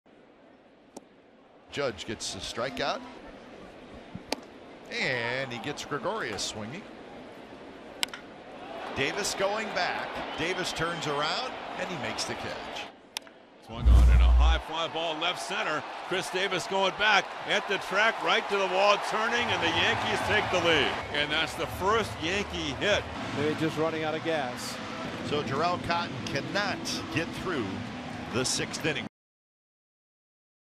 OAK@NYY: Cotton K's five, flirts with no-no
Jharel Cotton strikes out five batters over 5 2/3 innings and carries a no-hitter into the 6th inning